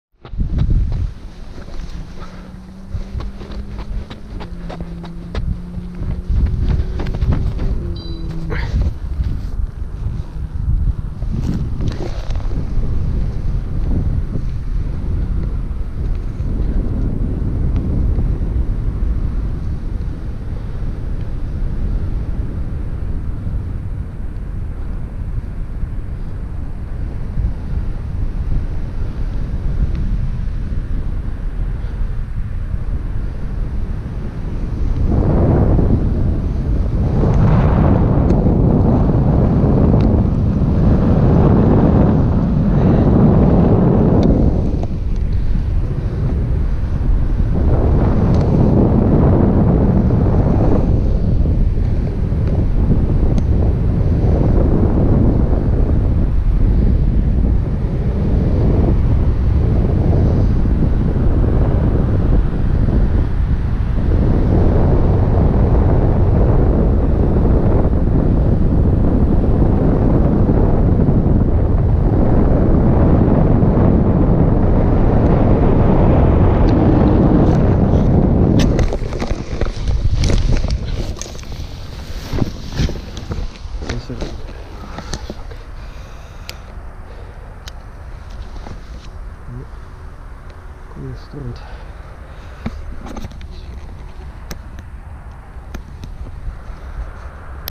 Terril du Roton paragliding 26 SEP 23
Paraglider flight from the south side of the slag heap of Terril du Roton in Farcienne. Wind coming from the south side.